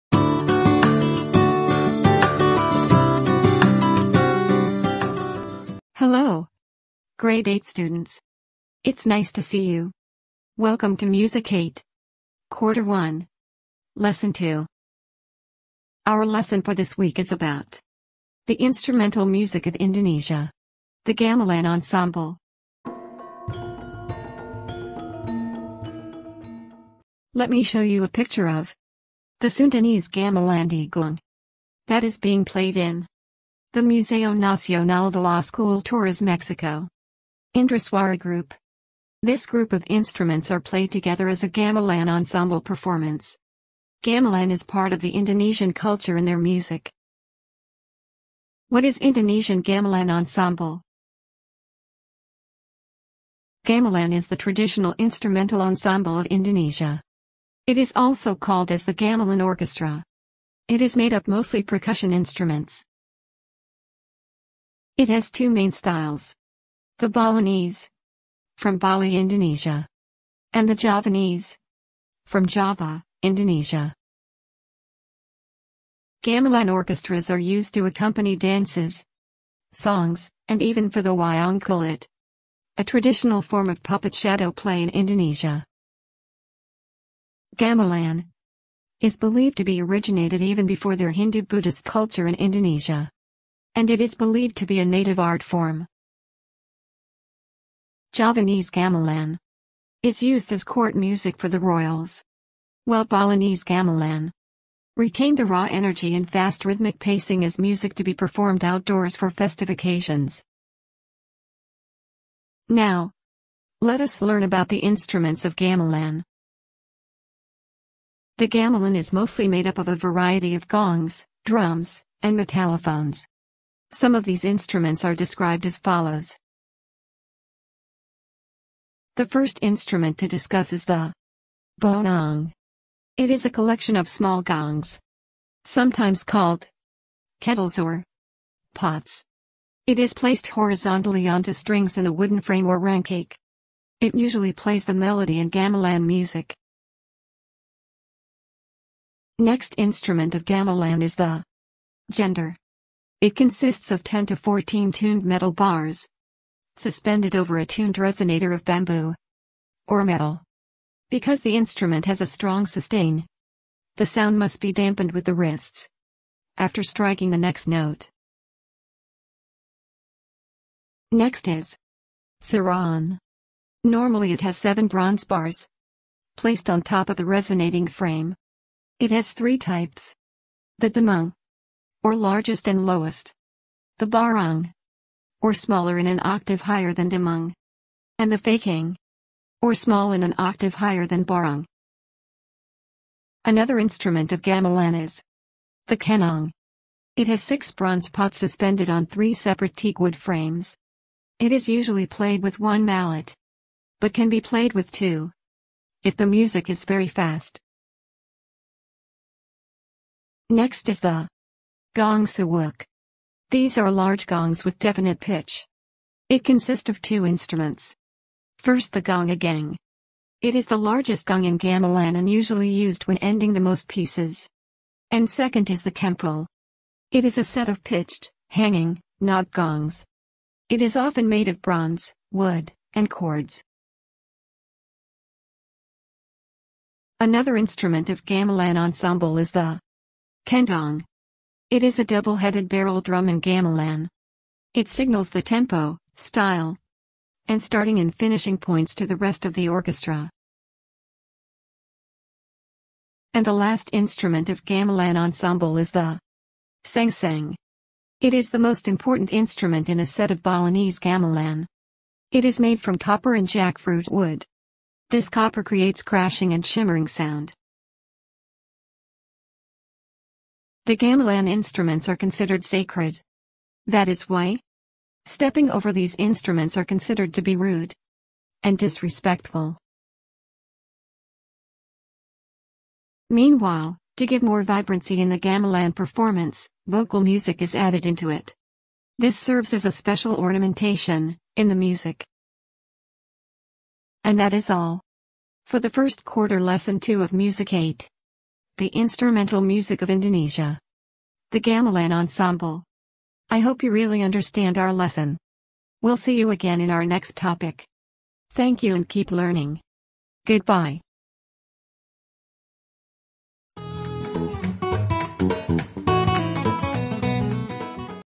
MUSIC 8 | LESSON 2 | INSTRUMENTAL MUSIC OF INDONESIA - GAMELAN ENSEMBLE | FIRST QUARTER
MUSIC 8 - FIRST QUARTER - WEEK 2: INSTRUMENTAL MUSIC OF INDONESIA | GAMELAN ENSEMBLE

Prepared by: Ms. Roxanne Lyn L. Redrico

Content: Based mainly on DepEd MUSIC 8 ADM
Reference:
- Anido, Belinda et.al. Music and Arts of Asia 8 Learner’s Module (2013), pp. 13-16
- Wikipedia contributors, "Music of Indonesia," Wikipedia, The Free Encyclopedia, (accessed May 24, 2020).
- Wikipedia contributors, "Pelog," Wikipedia, The Free Encyclopedia, (accessed May 24, 2020).
- National Music Museum contributors, “Kenong from the Javanese Gamelan Kyai Rengga Manis Everist”. (Most recent update:   April 3, 2014)
- Estrella, Espie. “Classification of Musical Instruments: the Sachs-Hornbostel System”, (Updated January 17, 2019)

THANK YOU for animaker.com for this wonderful video. My new buddy in Editing 😅